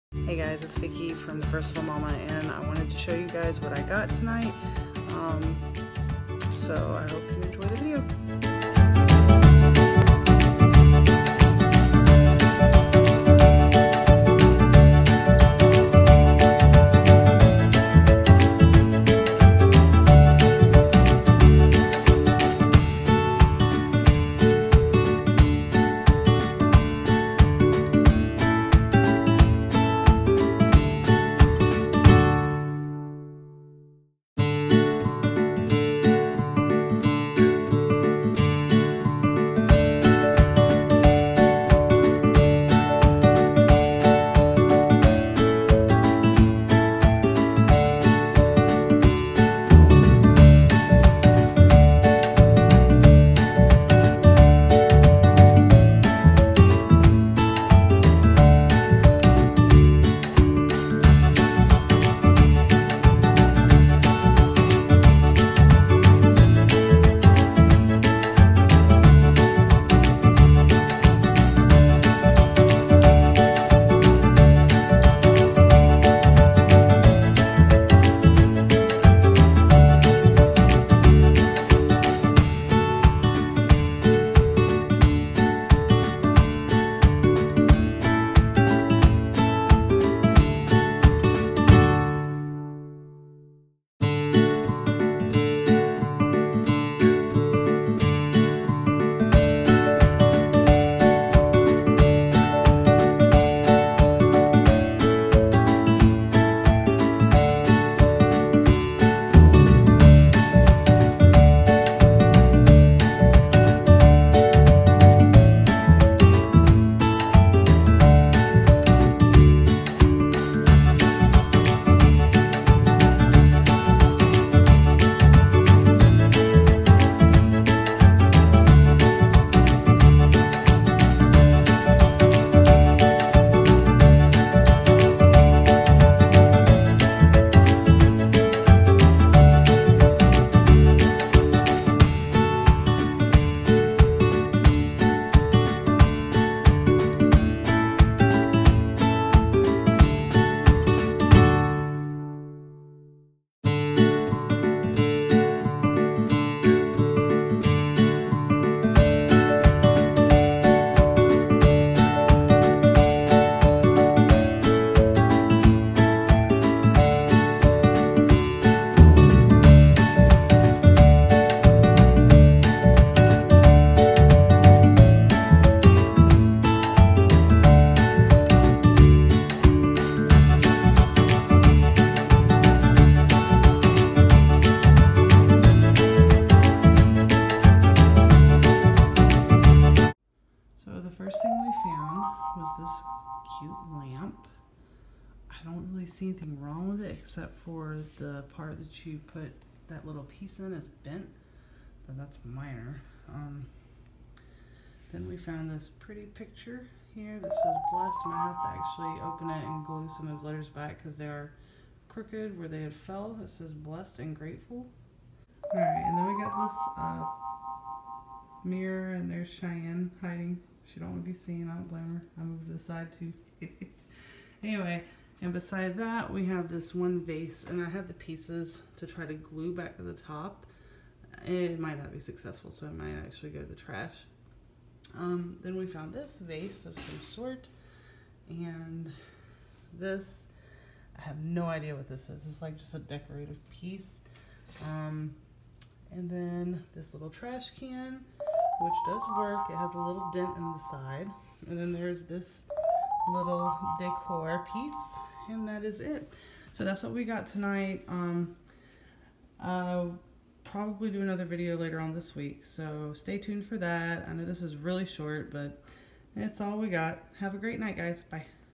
Kirkland’s Dumpster Dive And Haul $200 worth
We scored some really beautiful things tonight. Don’t forget to head to my website to enter the Victoria Secret giveaway!